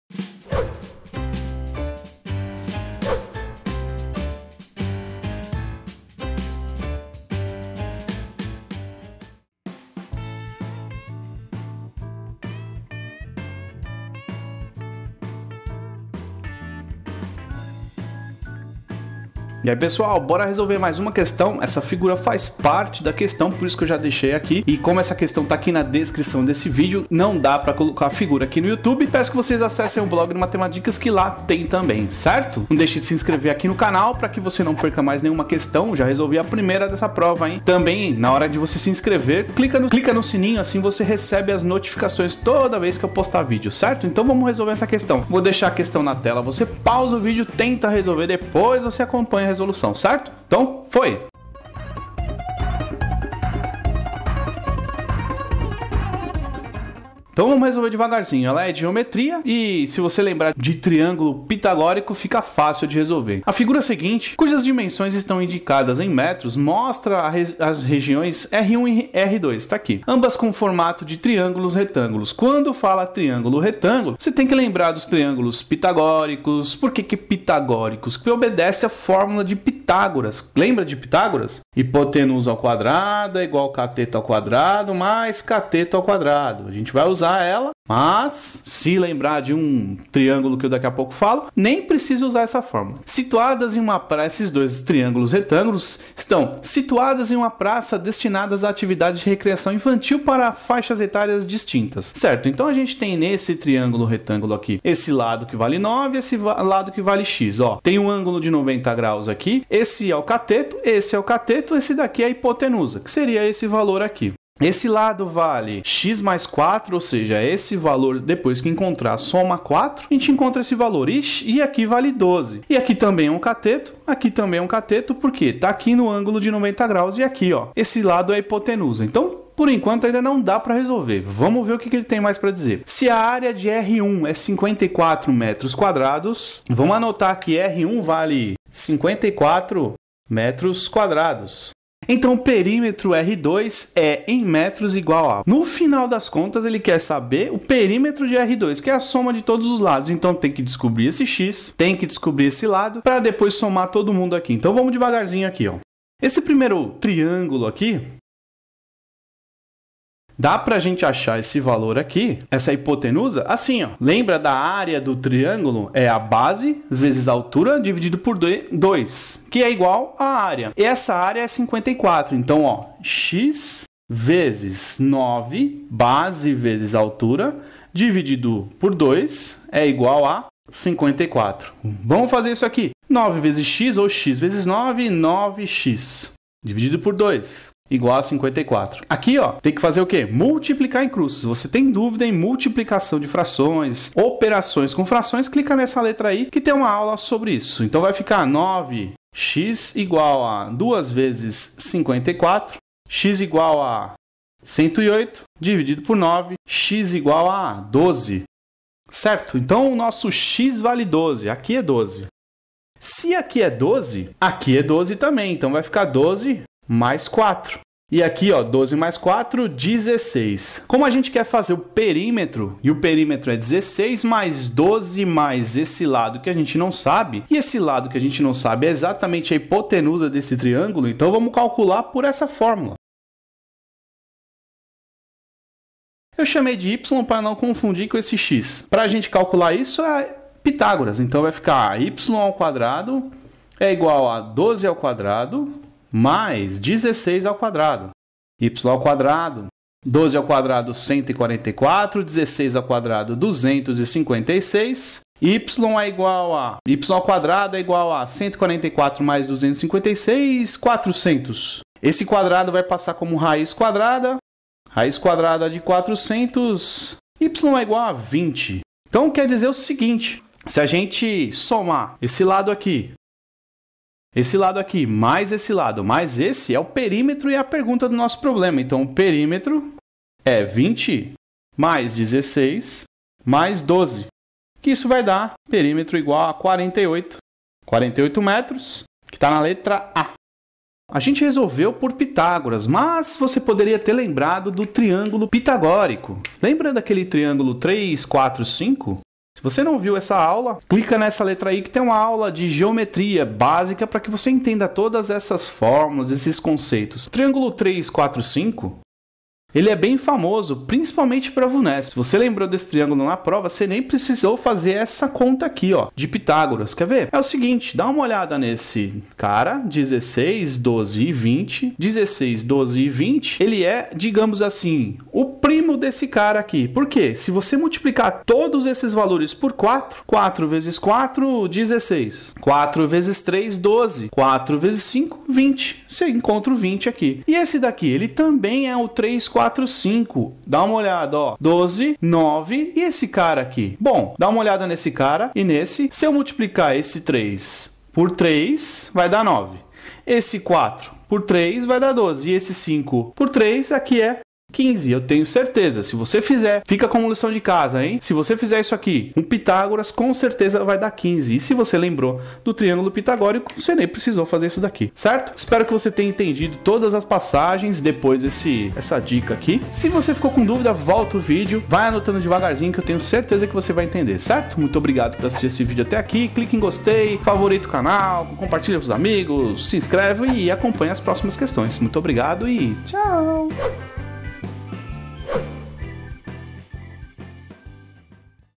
Geometria (02 de 06) – TJSP Capital – Vunesp 2017 – #0158 – Matemática
Questão - Vunesp 2017:
A figura seguinte, cujas dimensões estão indicadas em metros, mostra as regiões R1 e R2, ambas com formato de triângulos retângulos, situadas em uma praça e destinadas a atividades de recreação infantil para faixas etárias distintas.
[figura]
Se a área de R1 é 54 m², então o perímetro de R2 é, em metros, igual a
(A) 48.
(B) 36.
(D) 54.
(E) 40.